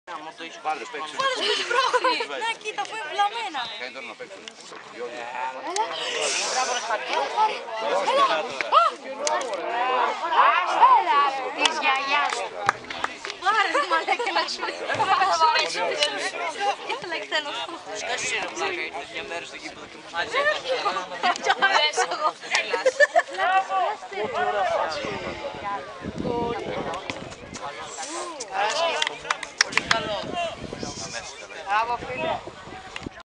Πελασγιακός - Ευρυτανικός 0-2, Φιλικός Αγώνας, Φάση 19η, Παρ 01 Σεπ 2017 DSCN5722
Φιλικός αγώνας μεταξύ ΑΟ Πελασγιακού  και ΑΣ Ευρυτανικού, ομάδα από την ανώτερη κατηγορία (Α' Φθιώτιδας), που διεξήχθη στο γήπεδο Πελασγίας, το απόγευμα της Παρασκευής 01 Σεπ 2017, με σκόρ 0-1 στο ημίχρονο και 0-2 τελικό.